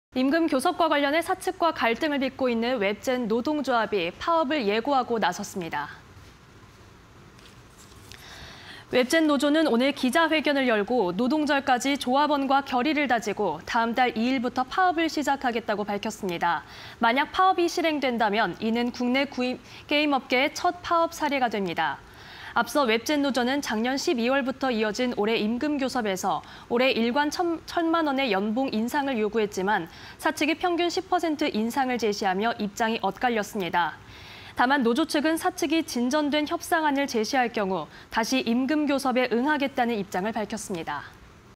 웹젠 노조, 내달 2일 파업돌입 예고... 국내 게임업계 최초(20220418)
웹젠 노조, 내달 2일 파업돌입 예고... 국내 게임업계 최초